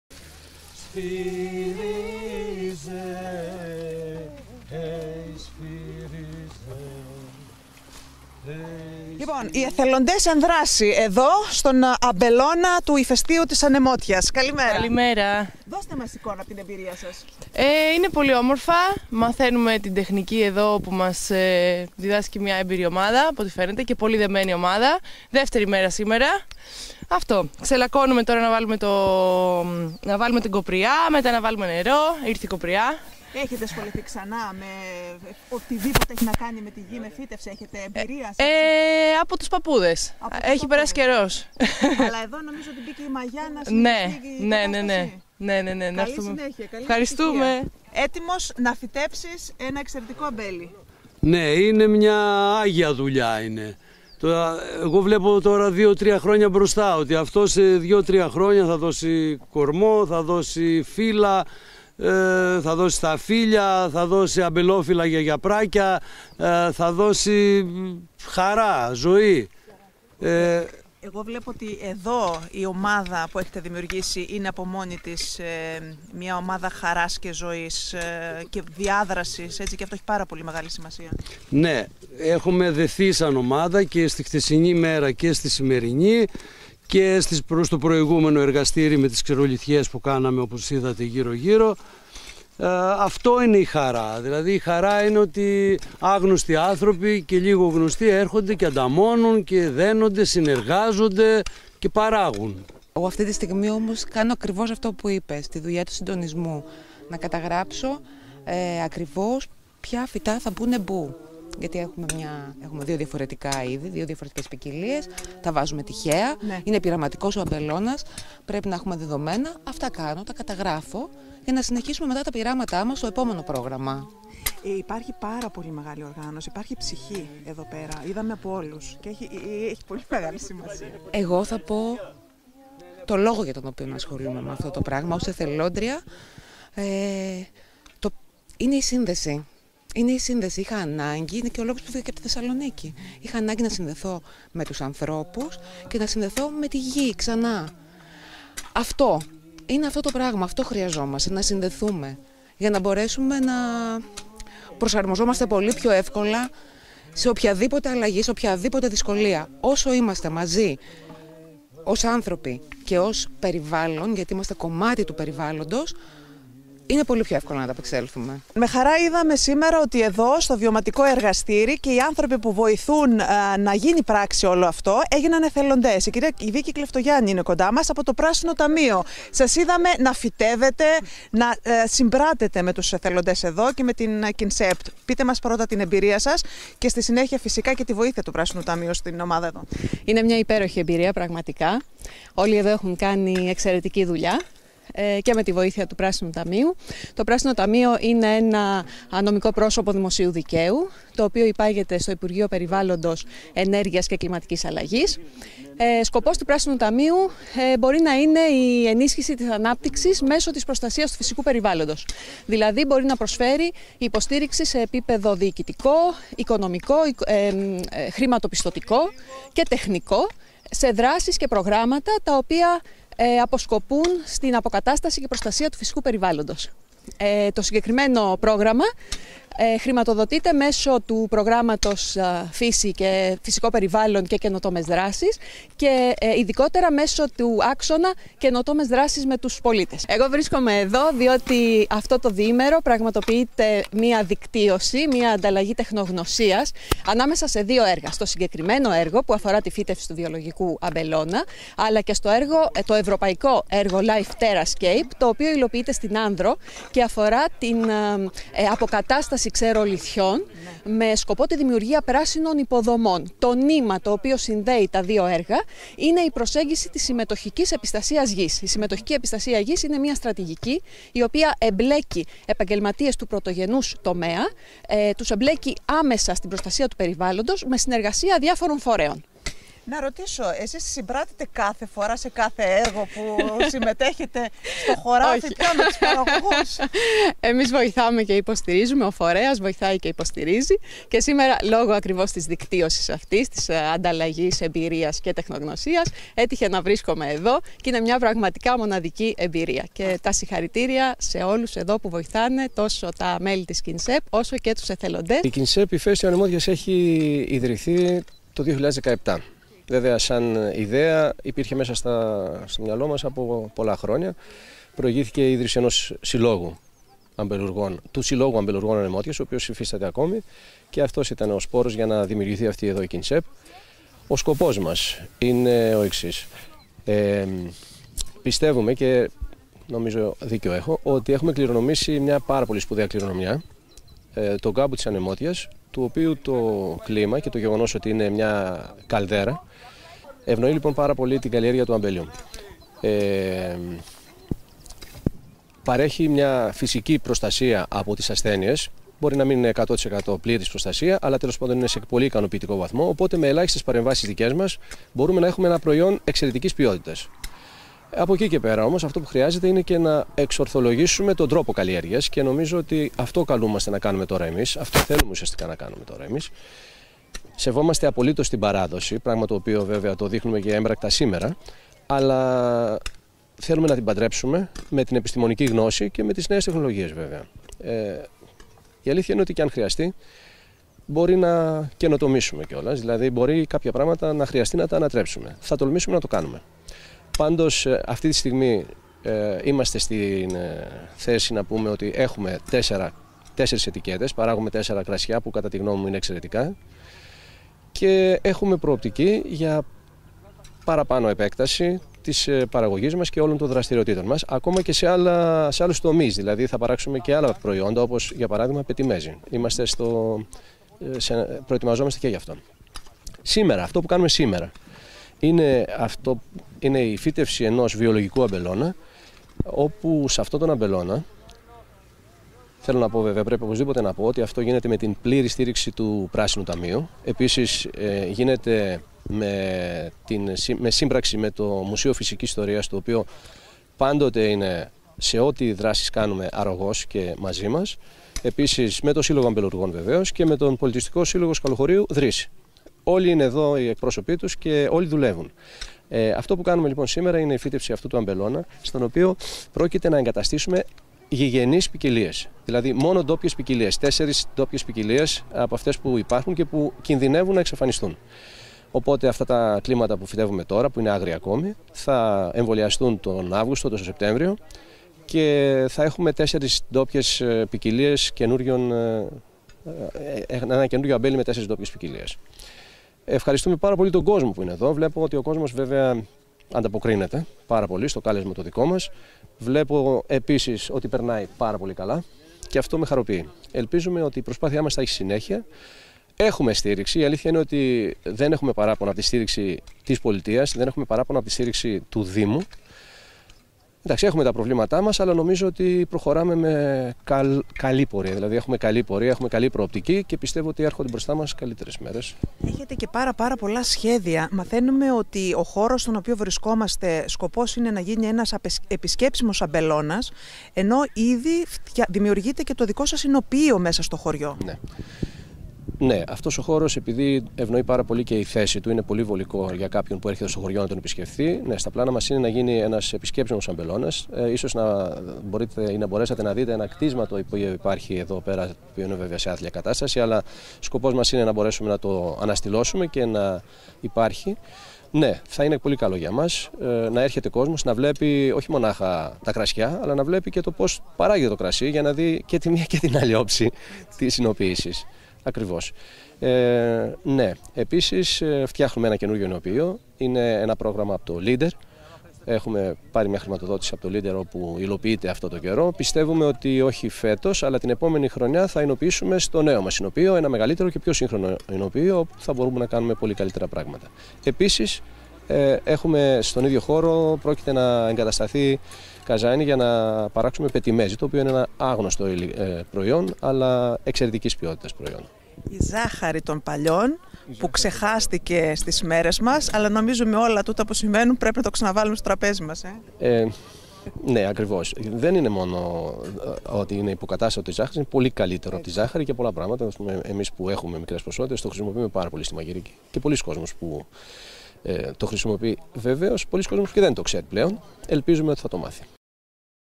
Βιολογικός αμπελώνας στον κρατήρα του ηφαιστείου της Ανεμότιας - Βιωματικό εργαστήρι φύτευσης
Βιωματικό εργαστήρι στη φύτευση βιολογικού αμπελώνα πραγματοποίησε το διήμερο 16 -17 Απριλίου η ΚΟΙΝΣΕΠΤ Ηφαίστειο Ανεμότιας. Η επιχείρηση προωθεί ένα σύγχρονο μοντέλο διαχείρισης της γεωργικής γης, ενώ η δράση αυτή αφορά την αποκατάσταση εδάφους και διάσωση γηγενών ποικιλιών αμπέλου και όλα αυτά, στον κάμπο της Ανεμότιας μέσα στον κρατήρα ενός από τα 4 ηφαίστεια της Λέσβου. Η ΚΟΙΝΣΕΠΤ Ηφαίστειο Ανεμότιας, έχει ήδη κυκλοφορήσει τέσσερις ετικέτες κρασιών  και ετοιμάζει το δικό της οινοποιείο. Το κτήμα που φυτεύτηκαν το διήμερο 1000 αμπέλια, προορίζεται να μετατραπεί σε έναν επισκέψιμο αμπελώνα.
Το διήμερο πάνω από 35 εθελοντές βοήθησαν να φυτευτούν 1000 άγρια κλίματα τα οποία το Σεπτέμβρη σε νέο βιωματικό σεμινάριο, θα εμβολιαστούν στις τέσσερις ντόπιες ποικιλίες σταφυλιών. Στο δημοτικό  σχολείο του χωριού που έχει παραχωρηθεί για τις δράσεις του συλλόγου αμπελουργών, το βιωματικό εργαστήρι συνεχίστηκε με παρουσιάσεις πάνω στην ιστορία και τη γεωμορφολογία της περιοχής αλλά και την τέχνη της αμπελουργίας.
Στην κάμερα της ΕΡΤ μίλησαν εθελοντές που συμμετείχαν στο βιωματικό σεμινάριο, η κ. Βίκυ Κλεφτογιάννη από το πράσινο ταμείο του υπουργείου περιβάλλοντος που χρηματοδοτεί το πρόγραμμα και ο Ηλίας Καστρινέλλης πρόεδρος της ΚΟΙΝΣΕΠΤ Ηφαίστειο Ανεμότιας.